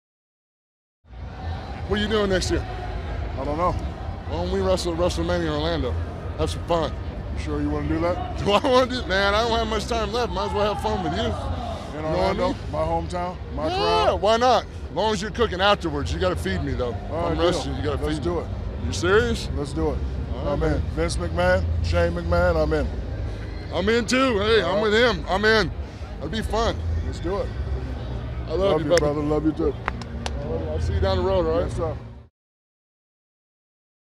Big Show challenges Shaq to a match at WrestleMania
The World's Largest Athlete lays down a huge challenge to Shaq on the red carpet of the 2016 ESPY Awards.